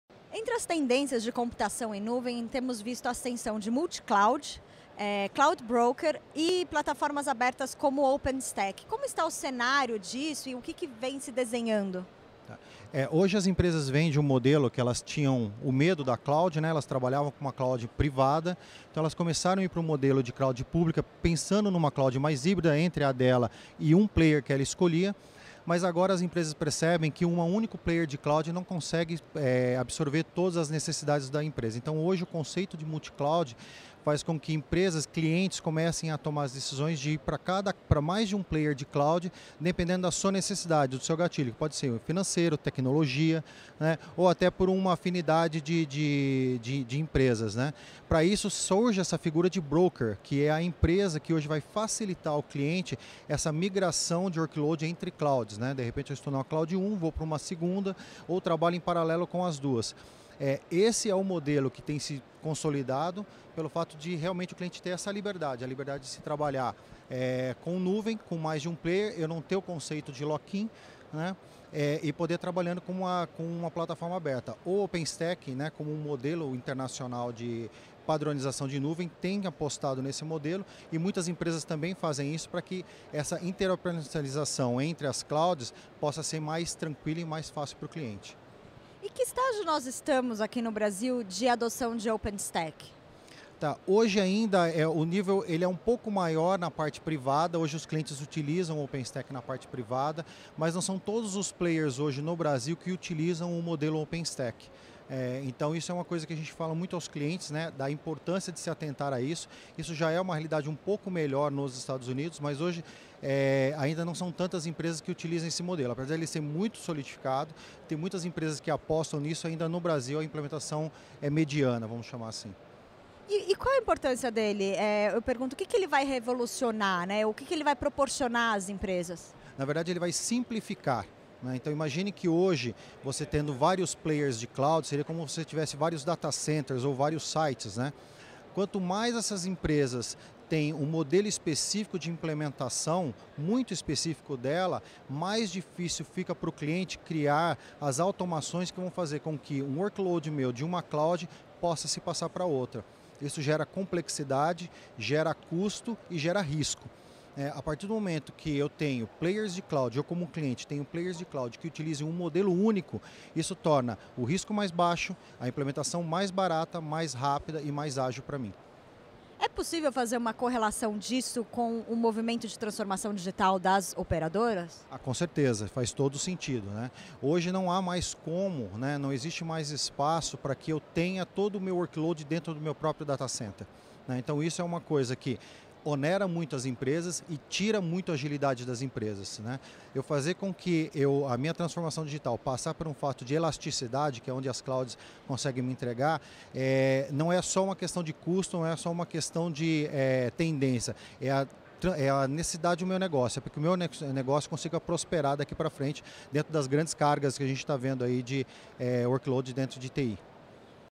OpenStack, multicloud e cloud broker lideram tendências da computação em nuvem
Multicloud, cloud broker e plataformas abertas como OpenStack são algumas das tendências em computação em nuvem. Em entrevista à CDTV, do portal Convergência Digital, durante o Futurecom 2018, Marco Ruiz, gerente de soluções da da Huawei, explicou que as companhias começaram pela nuvem privada e estão migrando para cloud híbrida e pública.